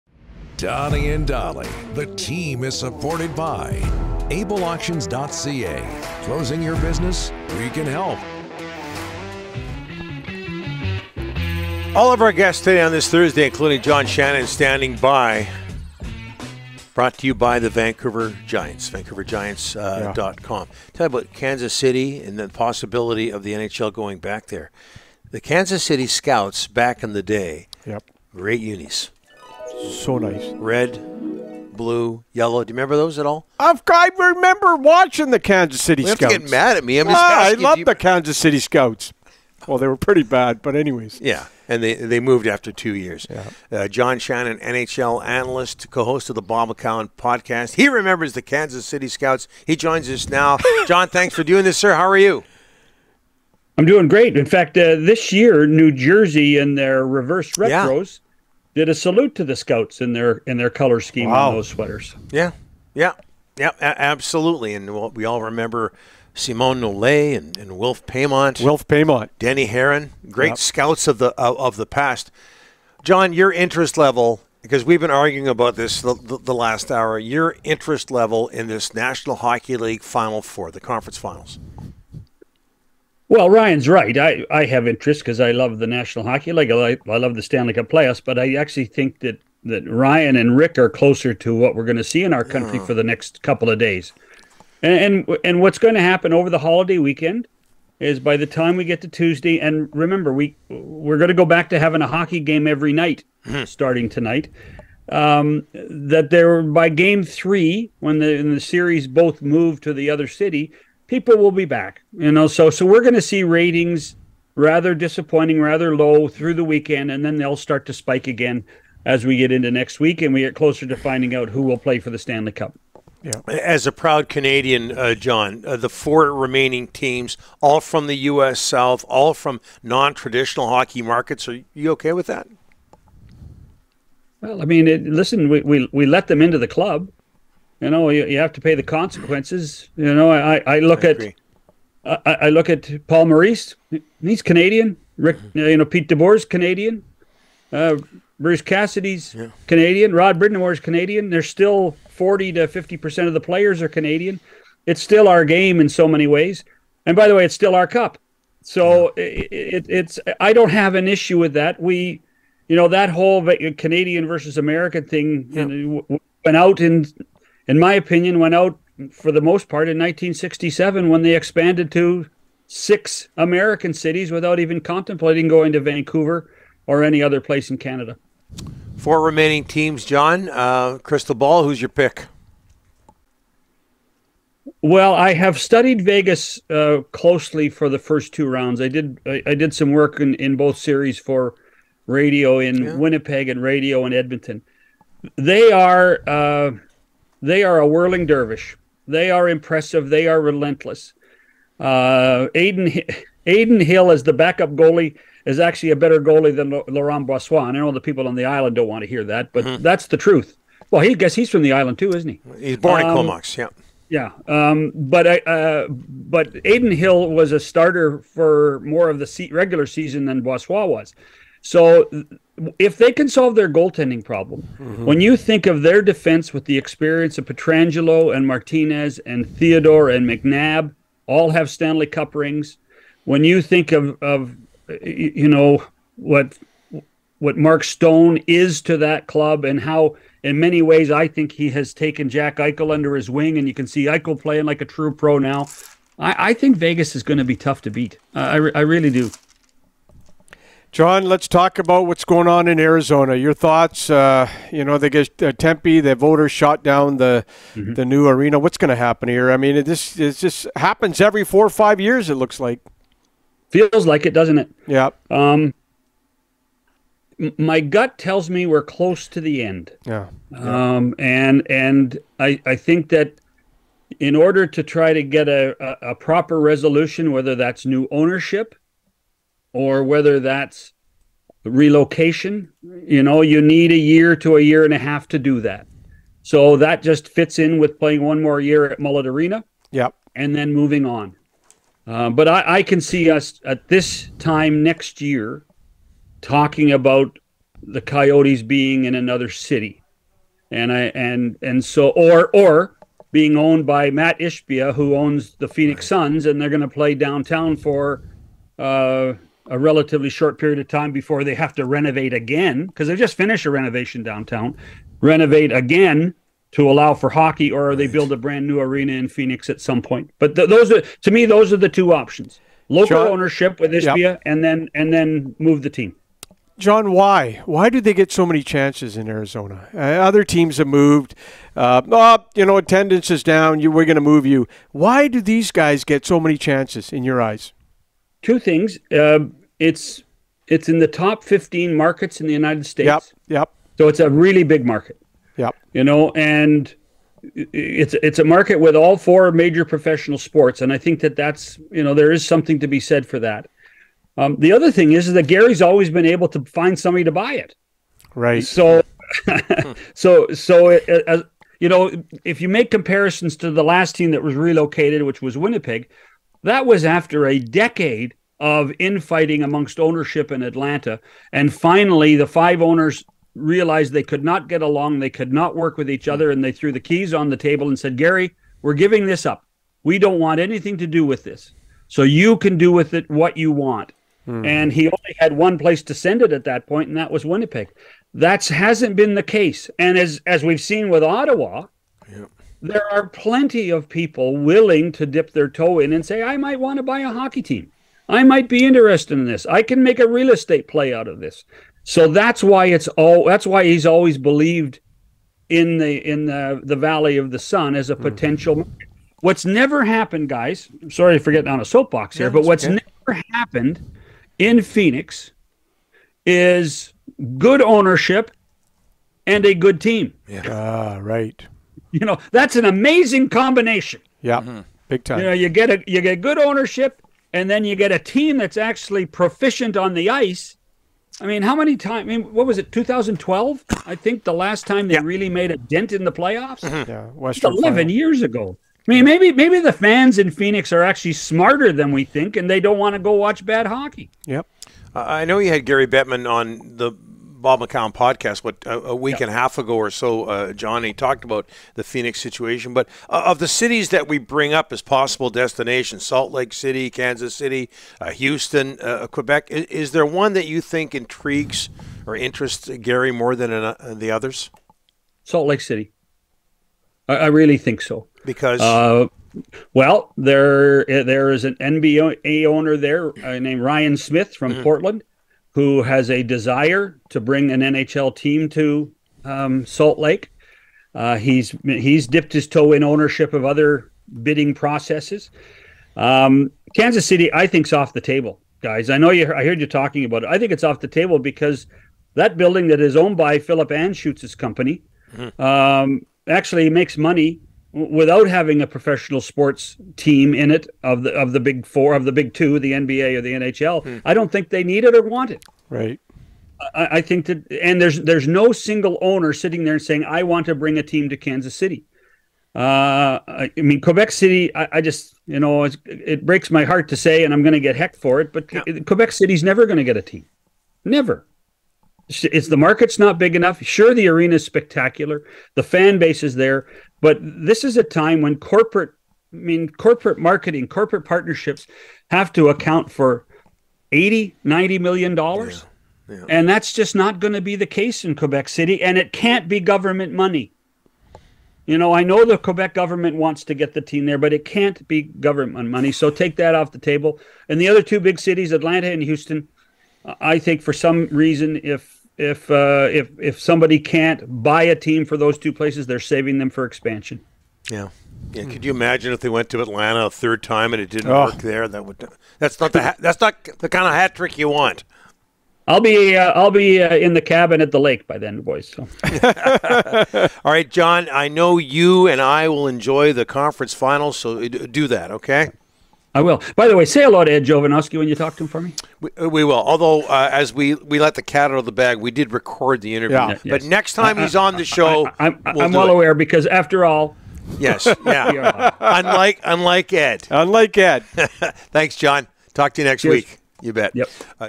John Shannon on interest level in the Conference Finals, the future of the Coyotes and more
NHL Analyst John Shannon on the interest level in the team remaining in the Conference Finals, the Coyotes future and more.